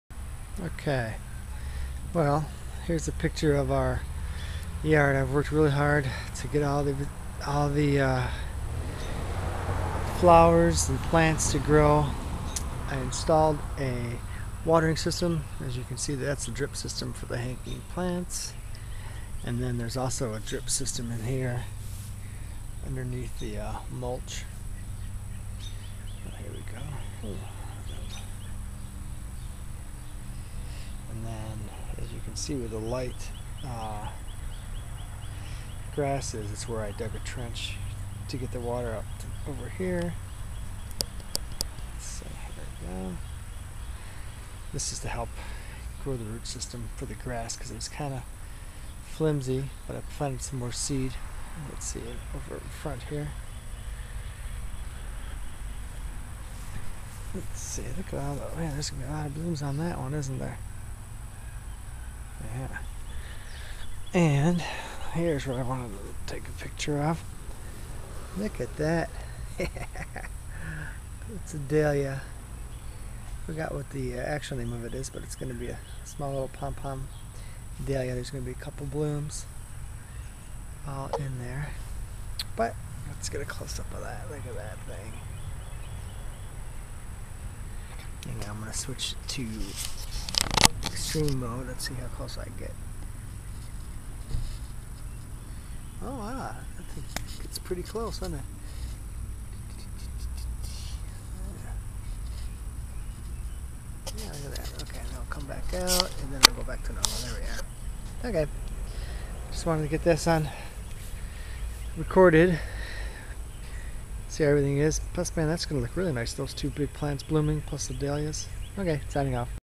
Garden plants blooming
I installed a garden watering system to help grow the plants in our front yard.  I also planted some Dahlia tubers that have started to bloom.